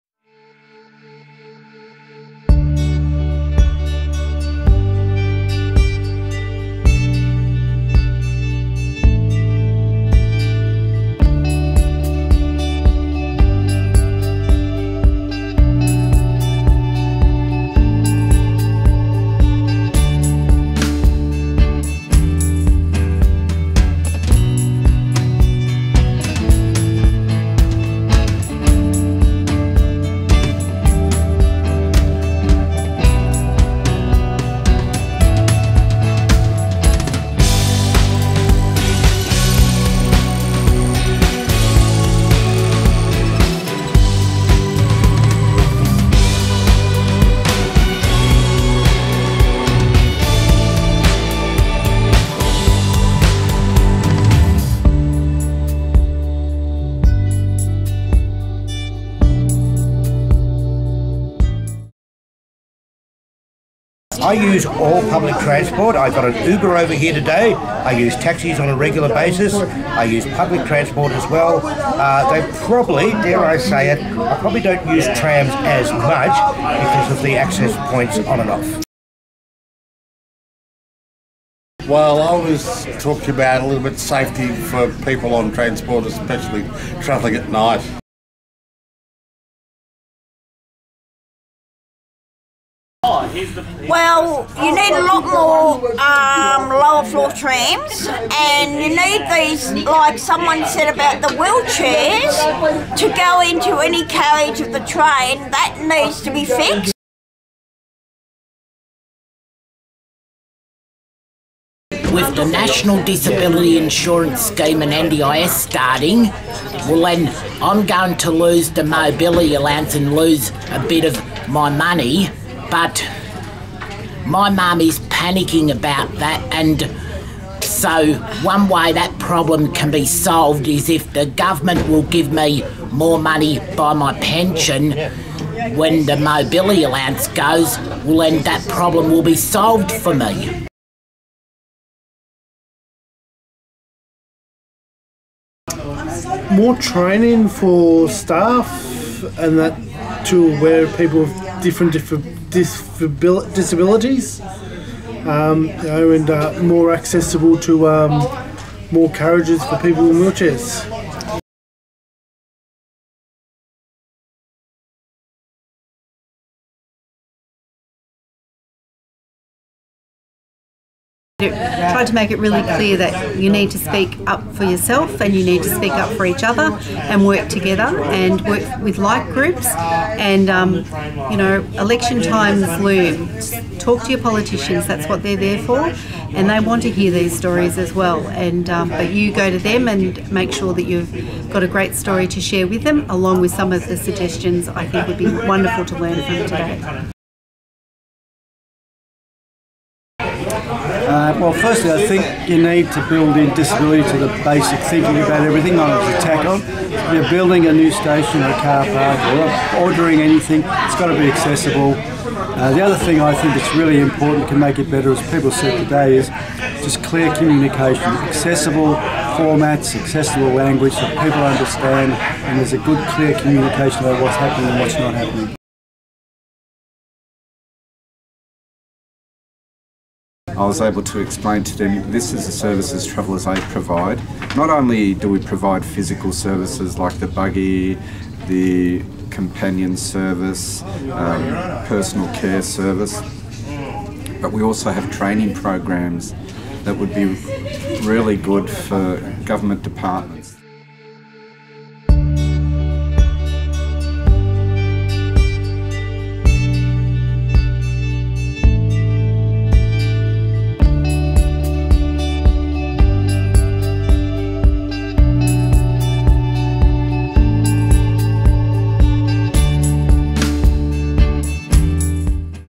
Victorian Self Advocacy Network Public Transport Forum 2017
In 2017 VSAN held a forum on public transport. The forum was attended by representatives from 12 self advocacy groups. This is a short feedback video filmed at the end of the forum.